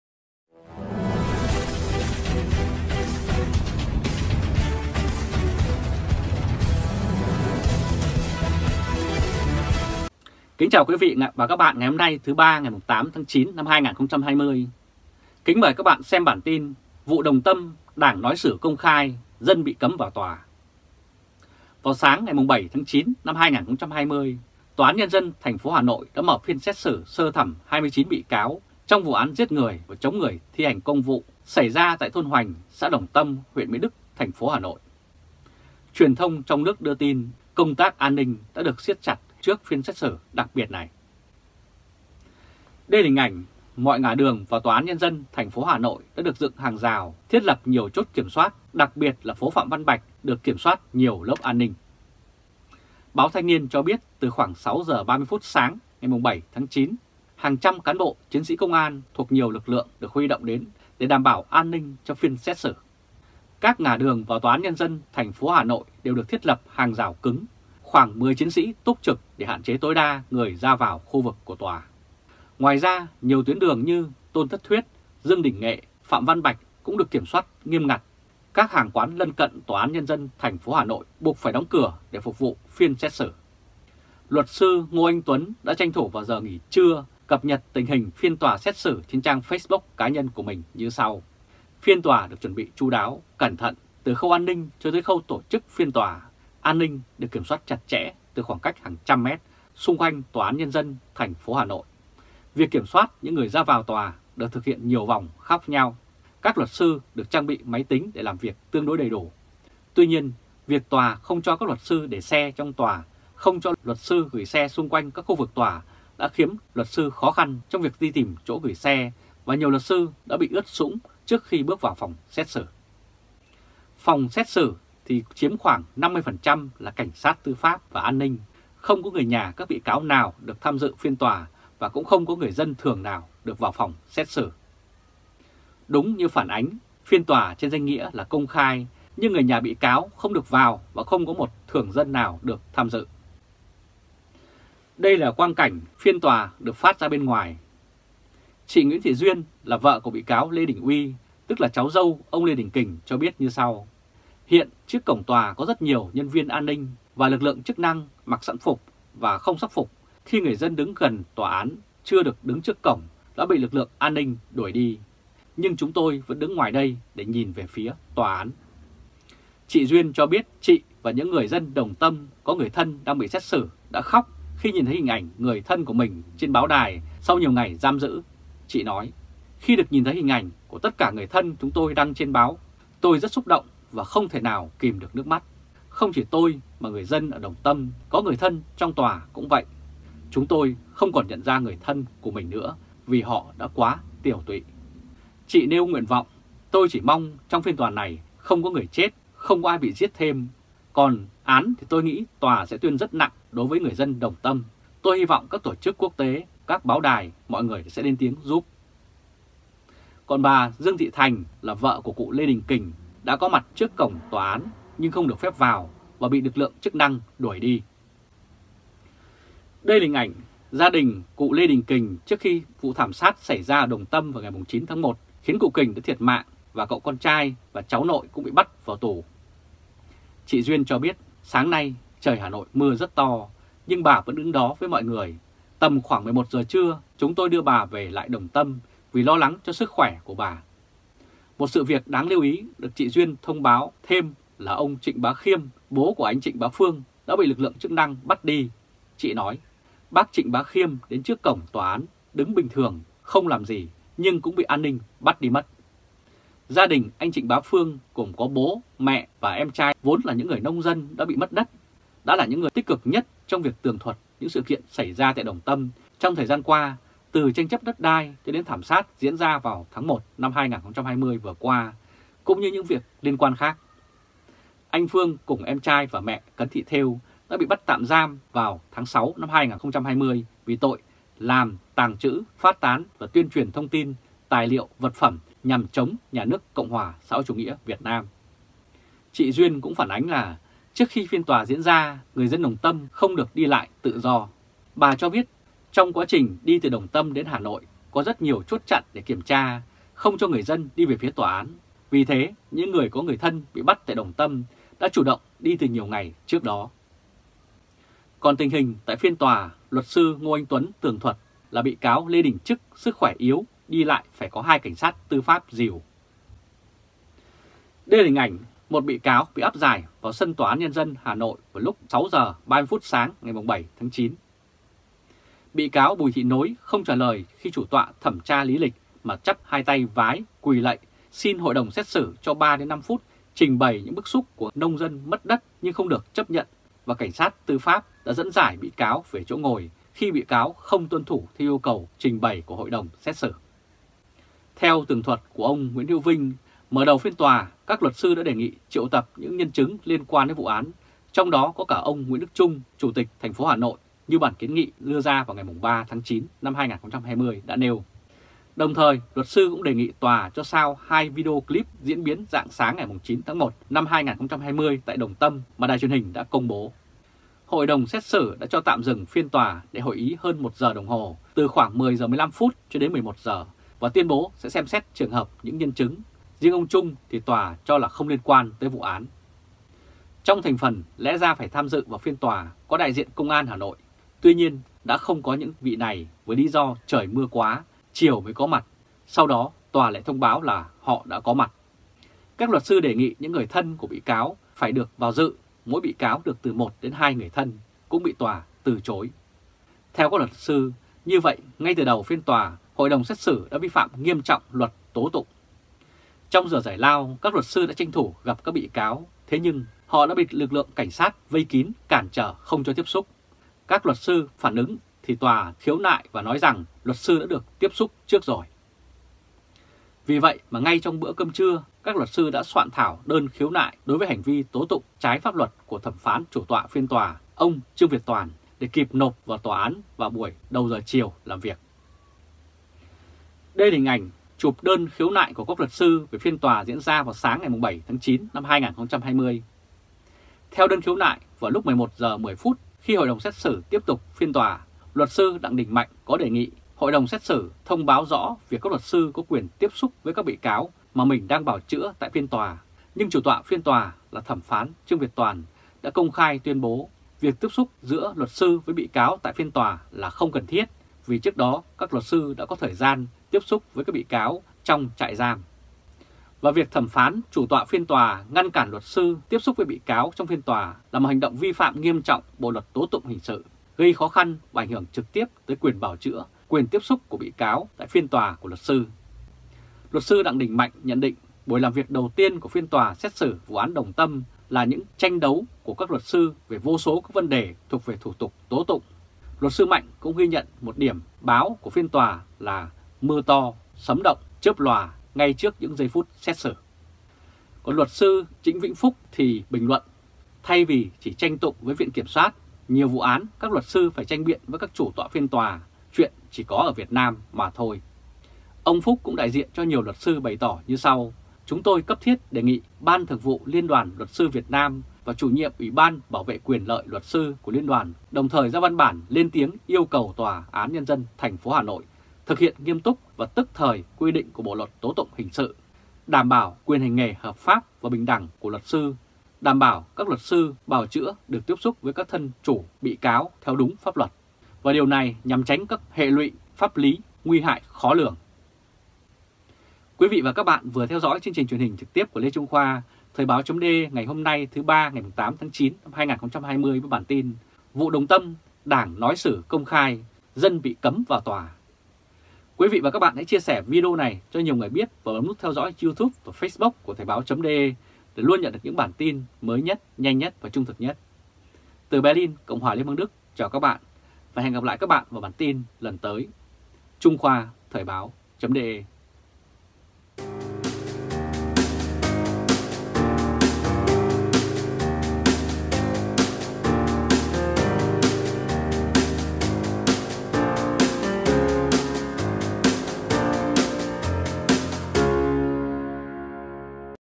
Vụ Đồng Tâm: Đảng nói xử công khai – Dân bị cấm vào Tòa
Lê Trung Khoa từ Berlin, CHLB Đức. / Bản tin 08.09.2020
Cách cài vượt tường lửa miễn phí: Điện thoại di động (mobile phone) = ; Trình duyệt (browser) =